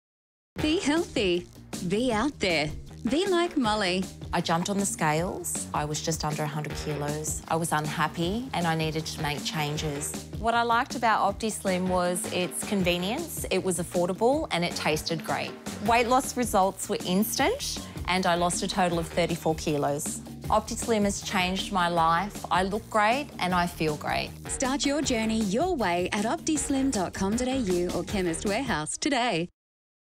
OPTISLIM Testimonials TVC | Optislim Review by Molly | Results in 2 Weeks
Welcome to our latest video where we feature real-life success stories of people who have tried OptiSlim and achieved incredible results. In this video, we feature Molly, who shares her personal experience with OptiSlim and how it has helped her lose weight and improve her overall health.

Molly talks about how she struggled with weight gain for years and tried various diets and exercise routines with little success. She explains how OptiSlim's meal replacement shakes helped her stay on track with her weight loss goals while also providing her with the necessary nutrients.

Molly also talks about the mental and emotional benefits she experienced with OptiSlim, including improved confidence and self-esteem. She shares review about the amazing transformation she achieved in just two weeks.

If you're looking for a weight loss solution that is effective, convenient, and can provide you with a boost in confidence, then OptiSlim meal replacement shakes are the right choice for you. Be sure to watch this video to hear Molly's story and learn more about how OptiSlim can help you achieve your weight loss goals.